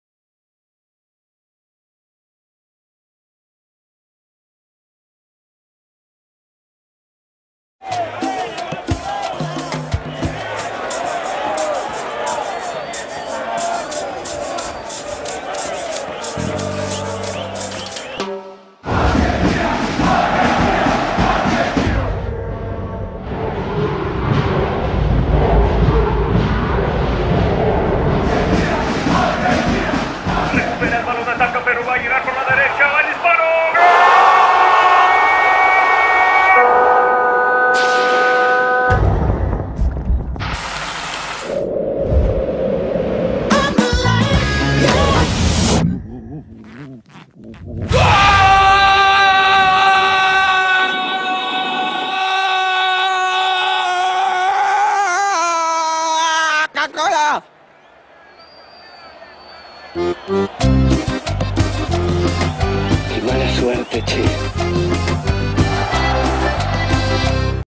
Coca-Cola Copa America - Infiltrado Peru
Publicidad de Coca Cola para la Copa America
Todos los derechos a sus respectivos creadores.
Version publicitaria para el Perú.